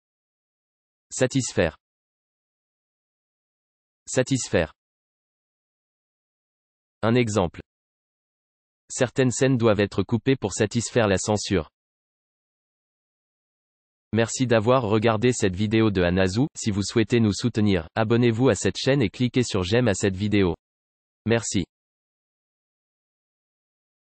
Comment prononcer «satisfaire» en français
Ça c'est comme on prononce «satisfaire». Êtes-vous surpris ou pas du tout étonnés?

Photo dans la miniature pris par «Valentin B. Kremer» sur unsplash